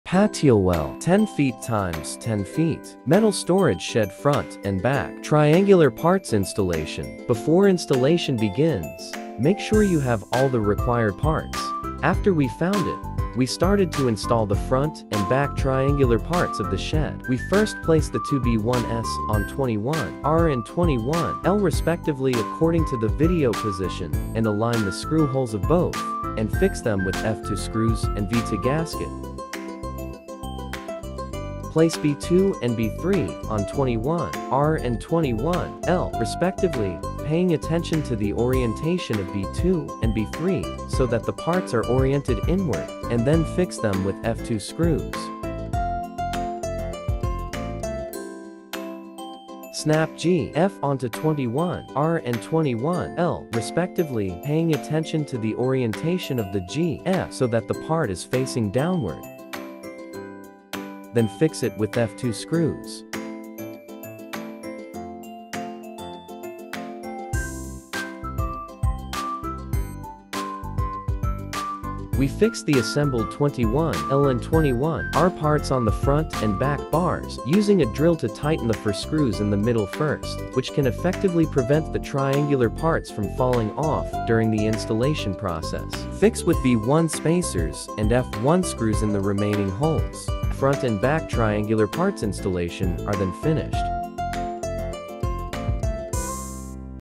10' x 10' FT Installation Of Triangular Panels - Easy Installation Steps
This is an assembly video for Patiowell 10' x 10' FT Storage Shed Installation Of Triangular Panels. There're some important steps you need to pay attention:
Step1, Place the two B1s on 21R and 21L respectively according to the video position and fix them
Step2, Place B2 and B3 on 21R and 21L and fix them
Step3, Snap GF onto 21R and 21L
Step4, Fix it with F2 screws
Step5, Fixed the assembled 21L and 21R parts on the front and back bars
Step6, Fix with V1 spacers and F1 screws in the remaining holes
Step7, Shake the triangular parts by hand to check if it is fastening